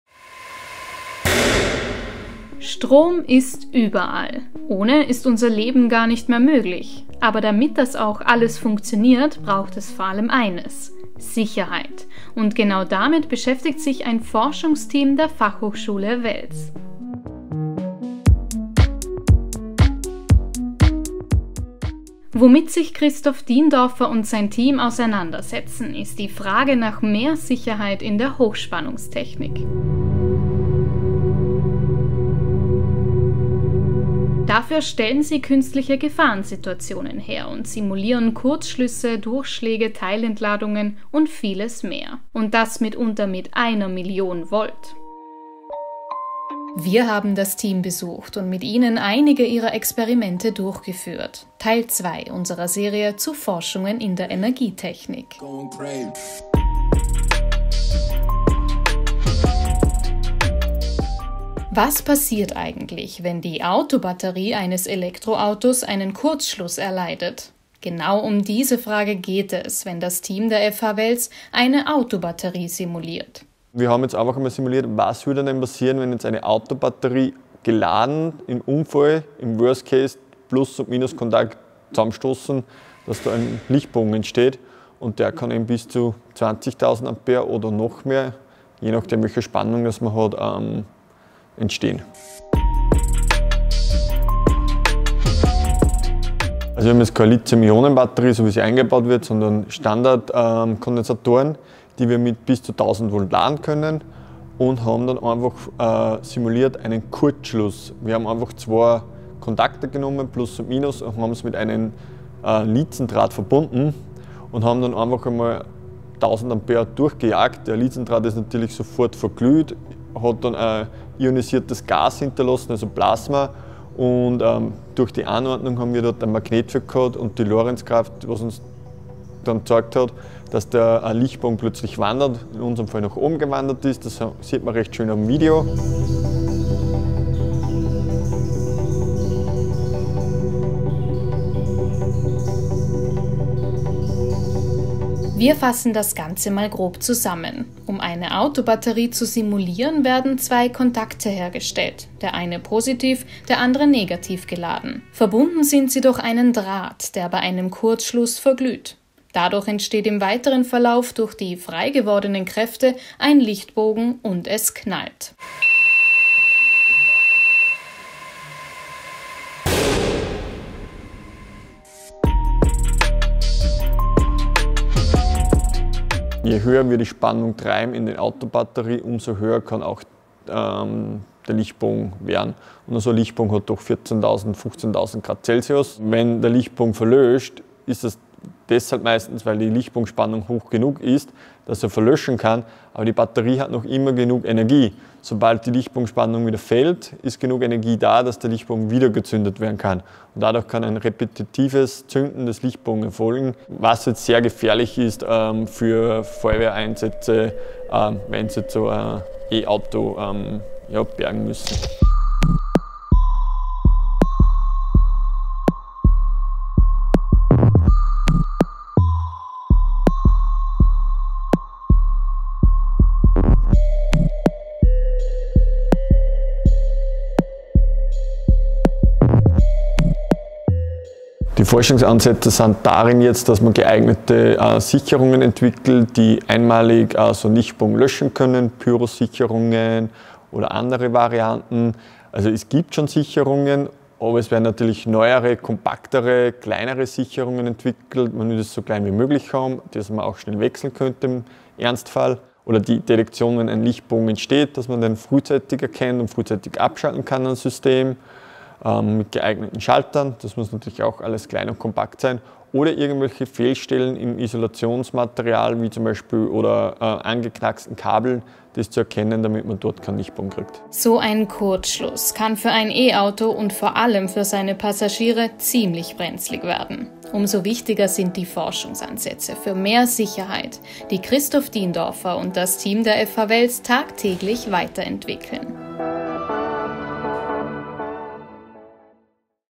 Elektroauto: Was bei einem Kurzschluss einer Autobatterie passiert | futurezone Reportage
Mehr Sicherheit in der Hochspannungstechnik: Damit beschäftigen sich Elektrotechniker Christoph Diendorfer und sein Team in ihrem Forschungslabor an der Fachhochschule Wels. Sie stellen künstliche Gefahrensituationen her und simulieren beispielsweise einen Kurzschluss einer Autobatterie. Dieses Szenario stellt für Elektrofahrzeuge eine erhebliche Gefahr dar.

Wir haben das Team an der FH Wels besucht und mit ihnen einige ihrer Experimente durchgeführt. Teil 2 unserer Serie zu Forschungen in der Energietechnik.

Unser Video gibt es auch auf unserem fuzo Watch Channel.